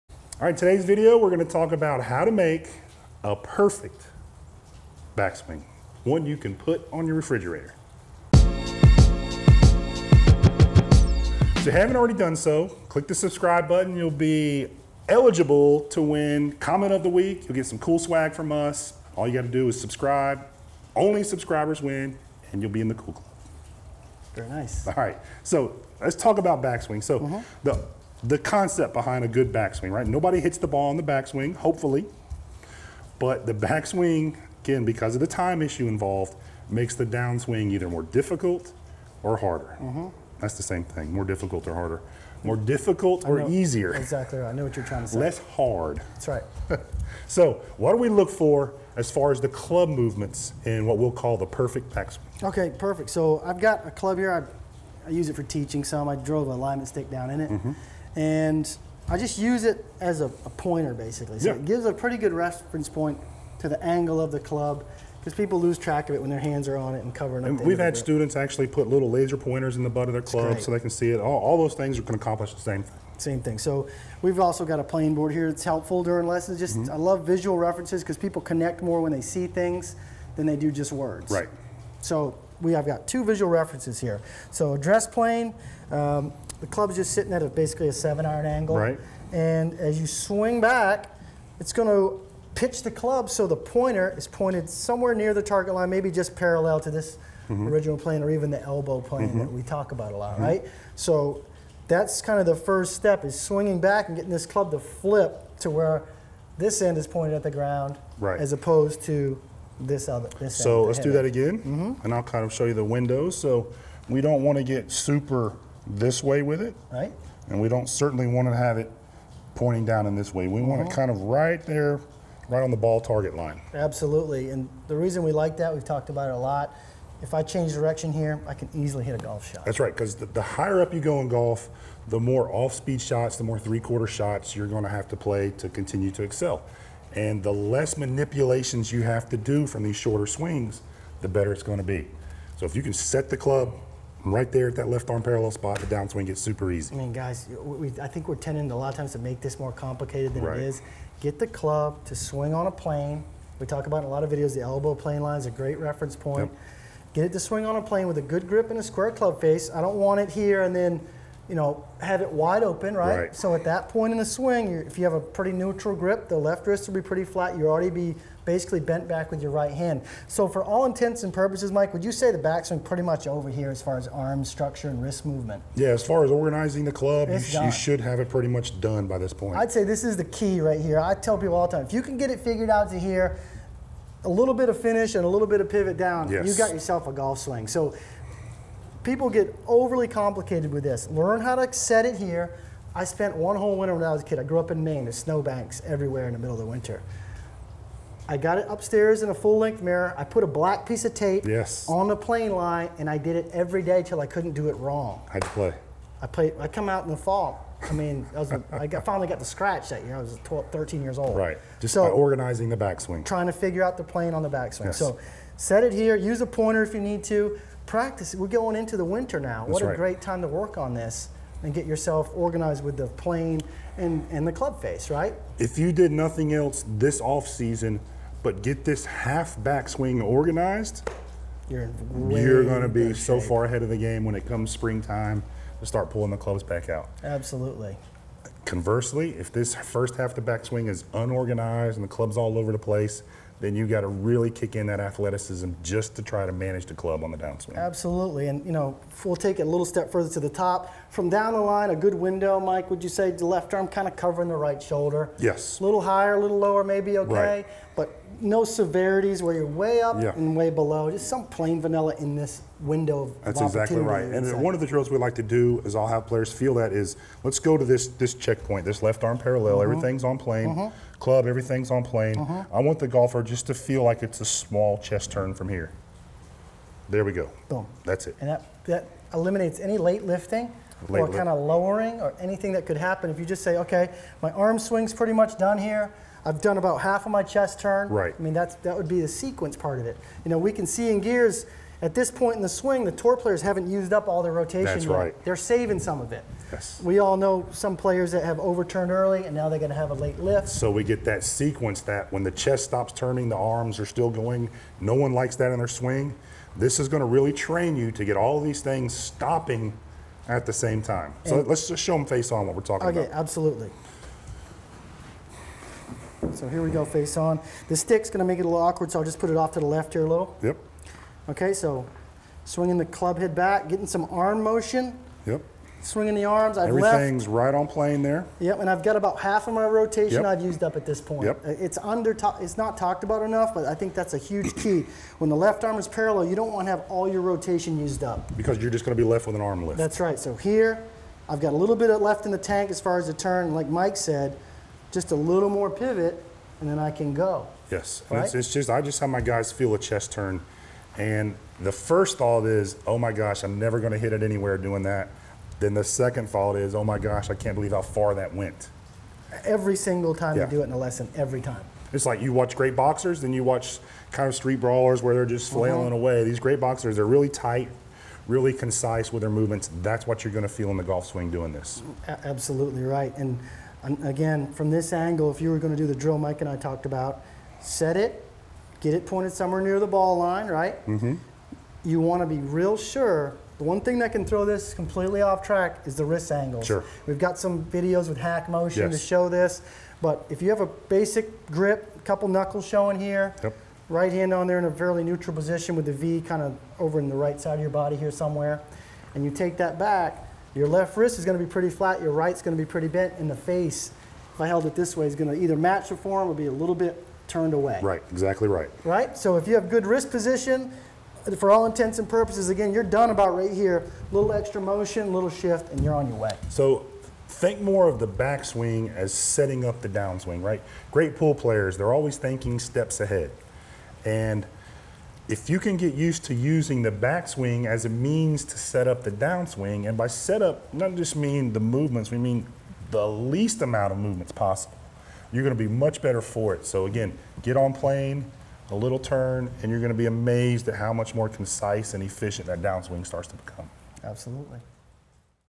How To Make A PROPER GOLF BACKSWING
A proper backswing should make your downswing EASY.  It should set up the downswing for all the shots, both partial and full.  This video will show you how to do it!